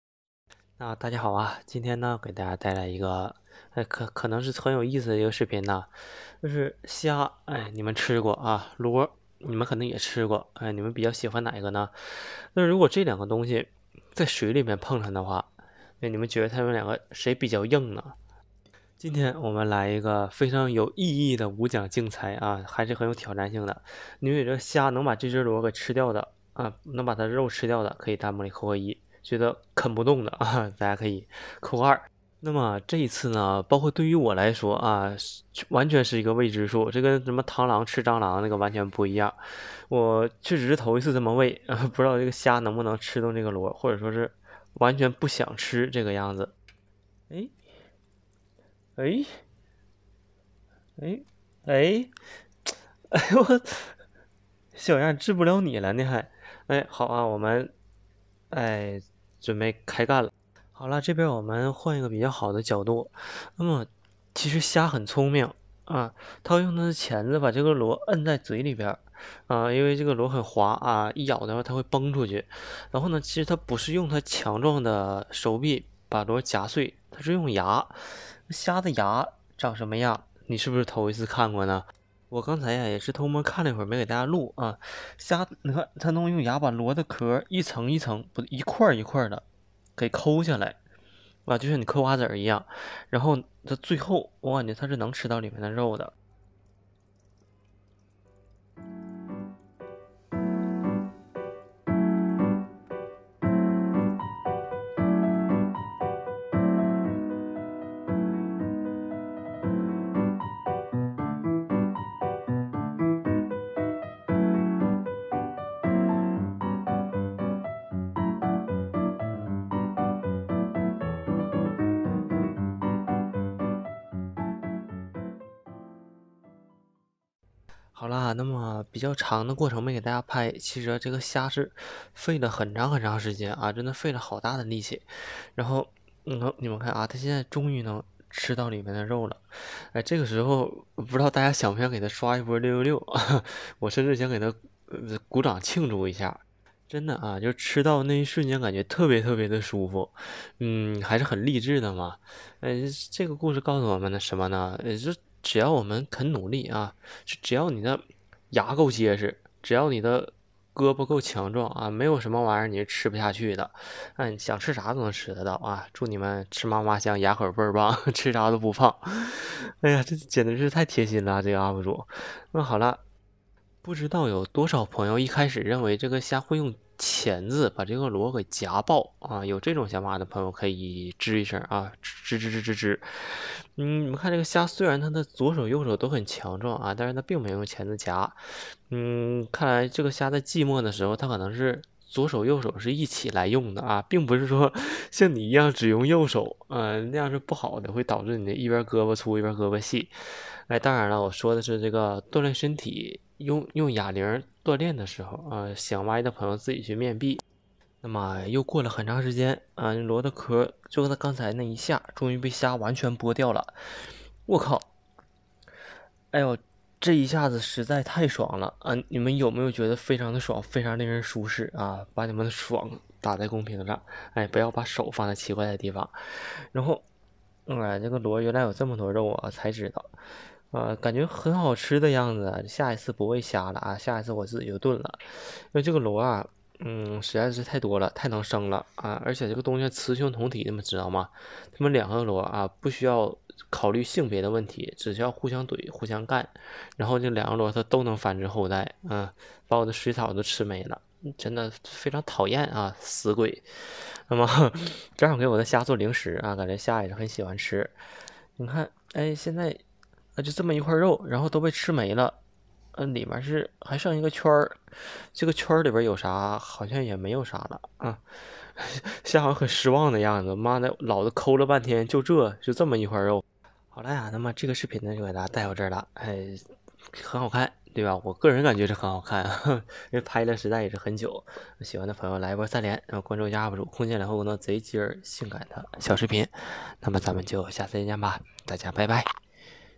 給藍蝦投餵一個螺！會發生什麼？【阿良良木翼】
扔給蝦一個螺，會發生什麼？它能吃到肉嗎？看誰更硬